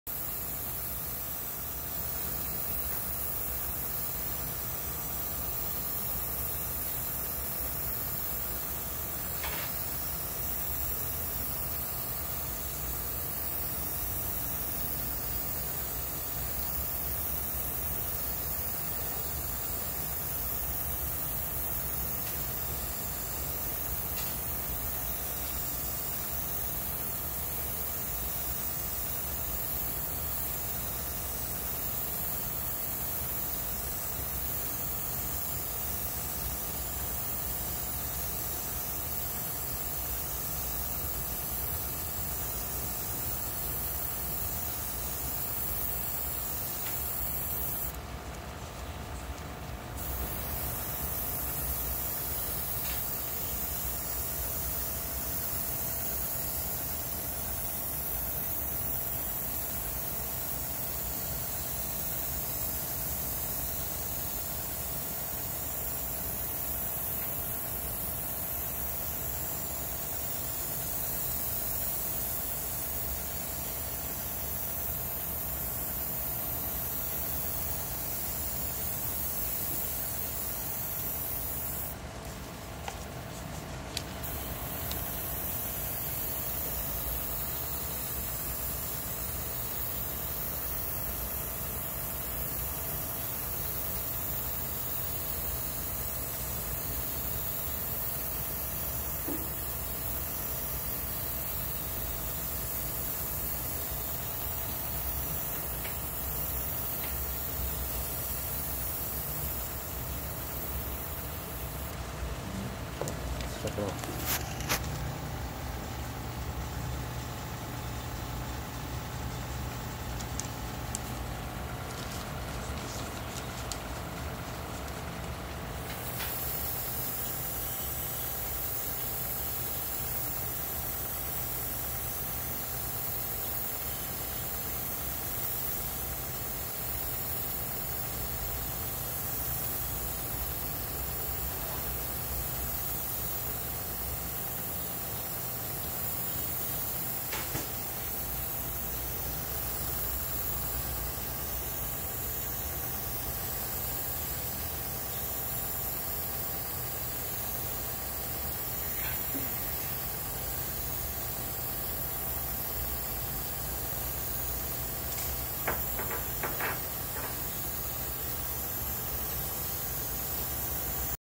튜닝타임즈 강좌 명동 정화예술대학교 평생교육원 박현준님의 Monogram 모노그램 1/25 Dodge Charger 다지 차저 1970 차체 색칠 작업
명동 정화예술대학교 평생교육원 프라모델 에어브러시 수업 수강생 박현준님이 흰색으로 사출된 차저의 차체를 색칠하고 있는데요, 워낙에 사이즈가 커서 픽업트럭과 견주어도 전혀 꿀리지 않을 크기라 시간이 더 오래 소요되고 있습니다^^